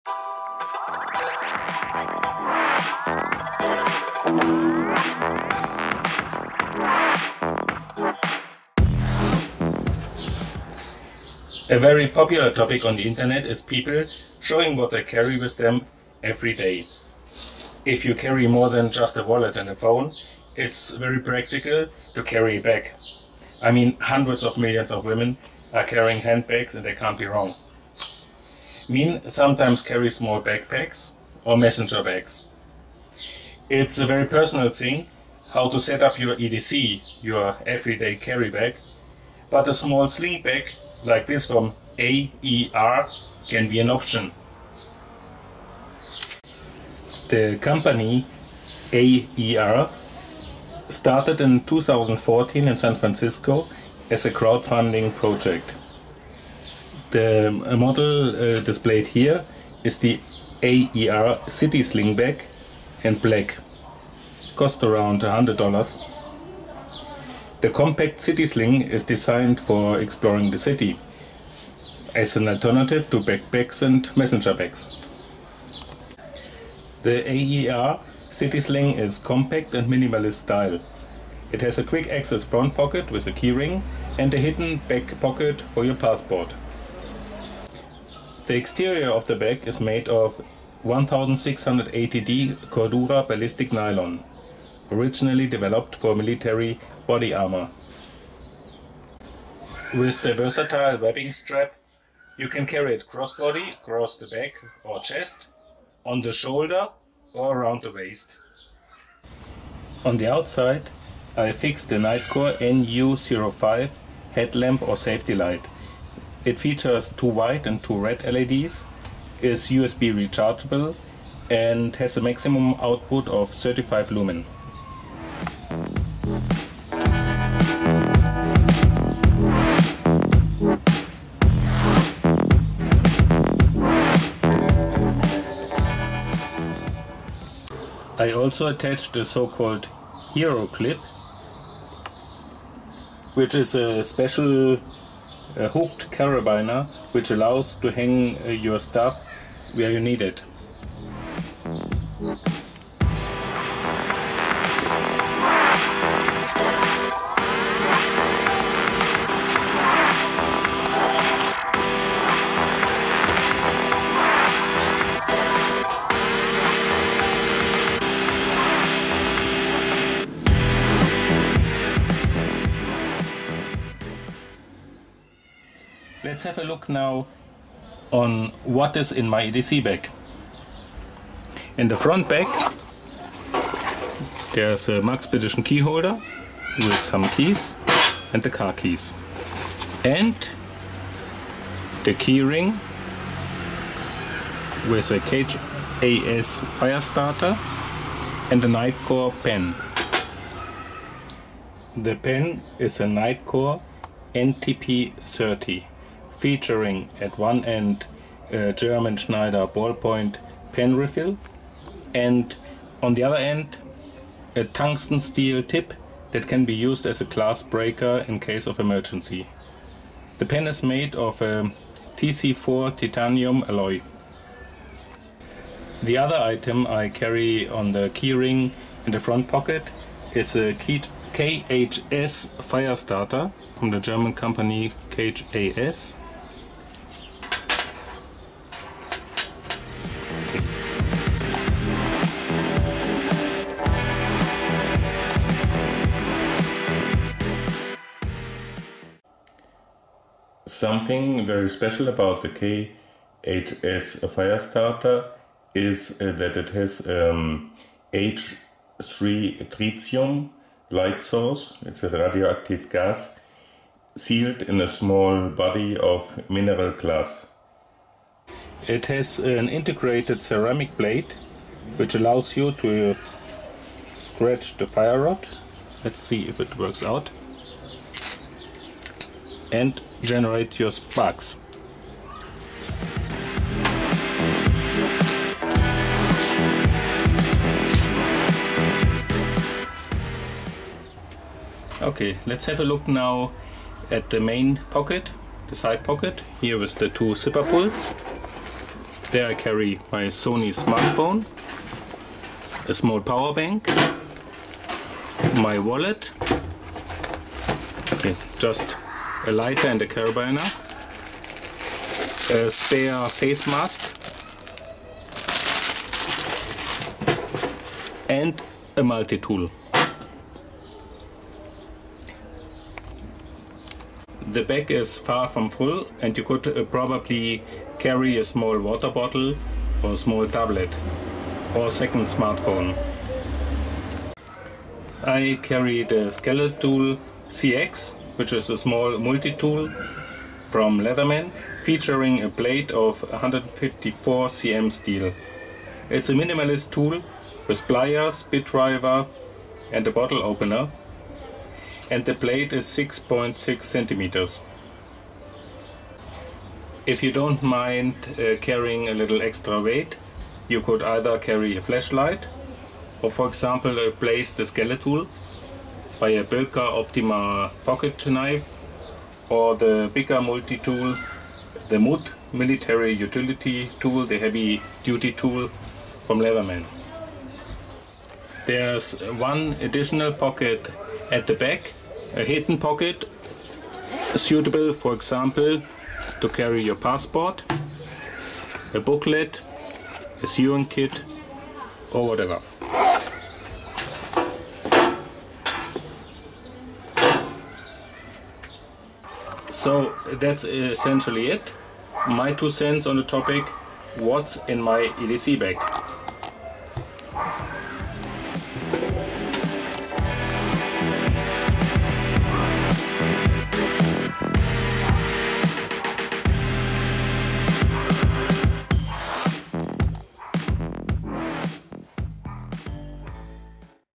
AER City sling bag / What is in my EDC bag? 2020
(English) (improved version)
featuring: Aer City slingbag, Nitecore Nu05 safety light, Heroclip carabiner, Nitecore NTP30 titanium pen, KHS Fire Starter with H3 Trigalight, Leatherman Skeletool CX multitool, Liefeventure Wallet, a 5000 maH power bank and my cat Anjali.